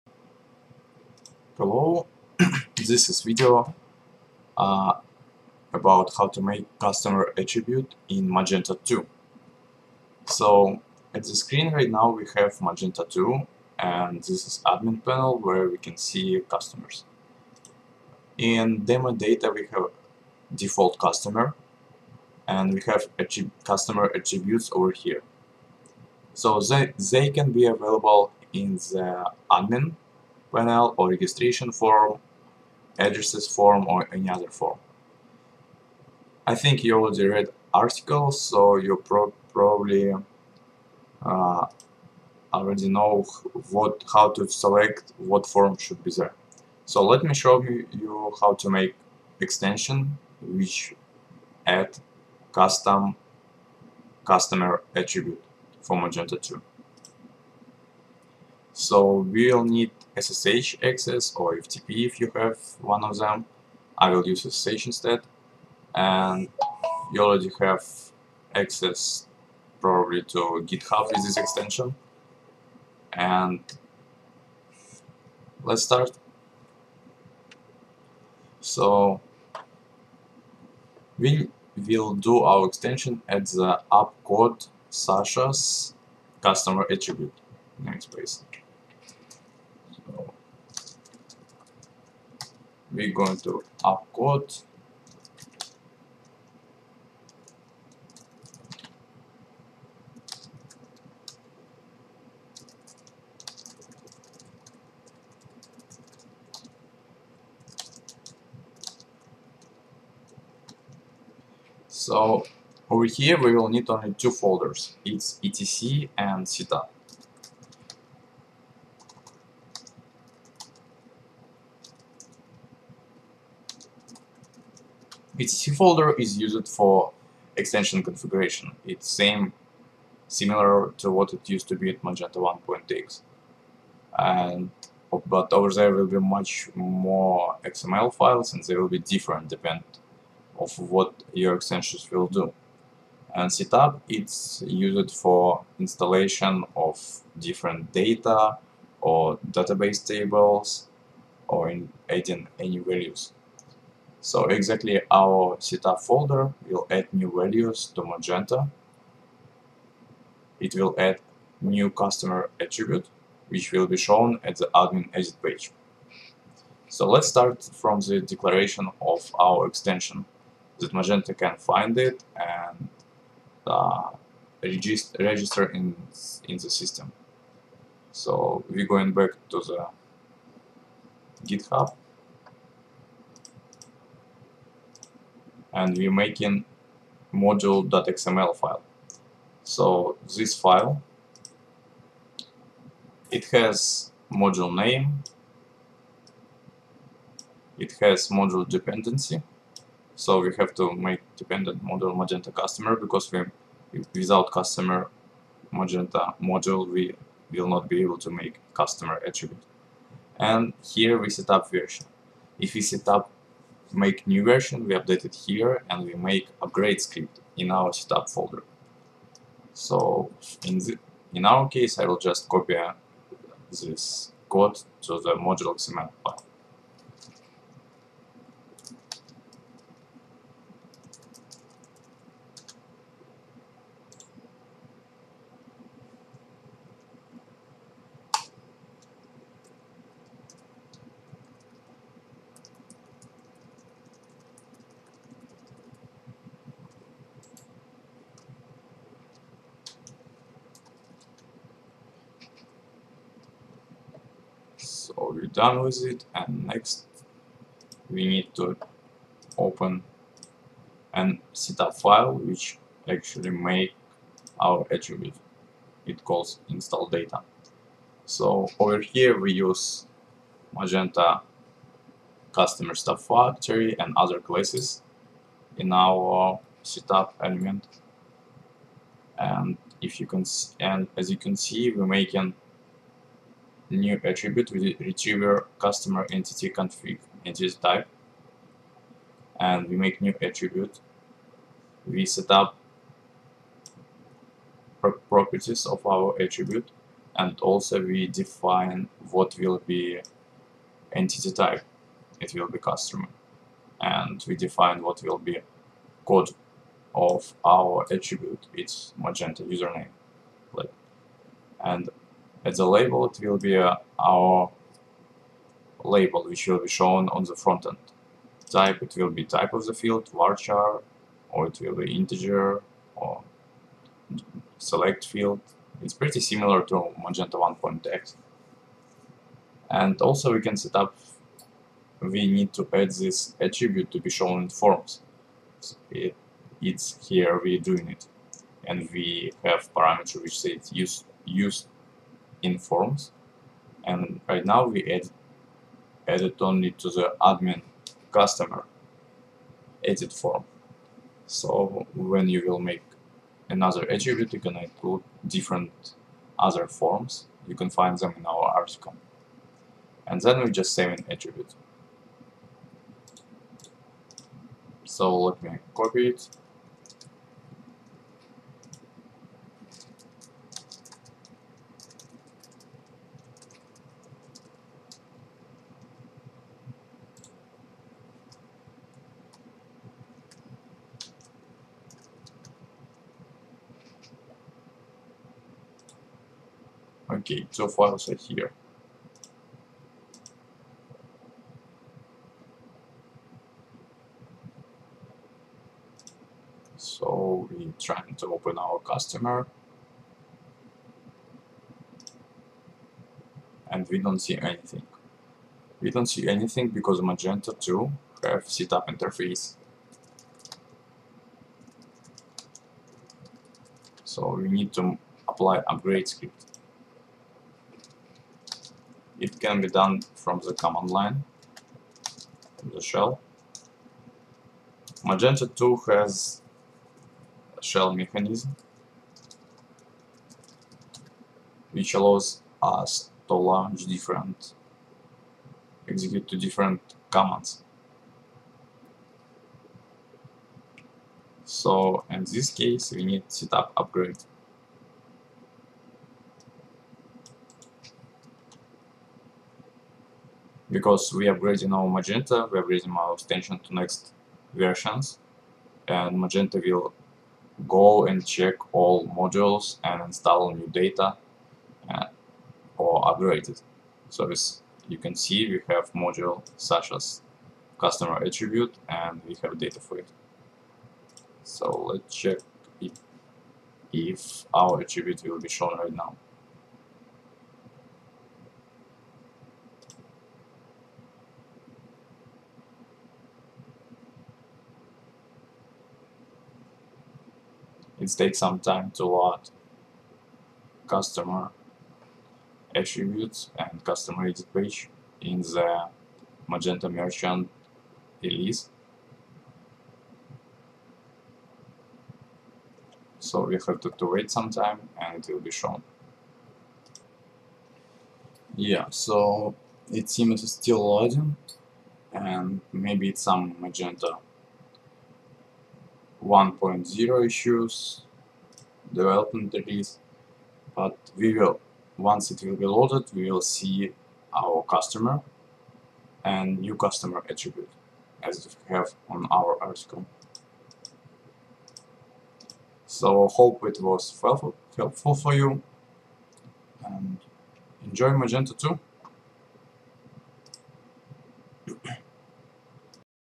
Magento 2: How To Make Customer Attribute ?
How to make new customer Attribute in Magento 2
The full article: